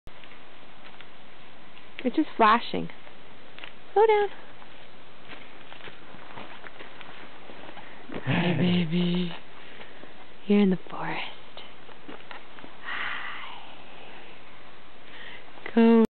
Hiking
One of our first hiking trips with the young lad